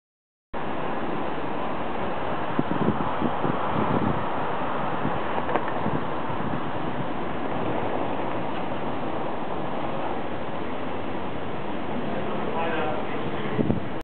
Yacht ECLIPSE
ECLIPSE passiert SAMAR in Hamburg 15-09-09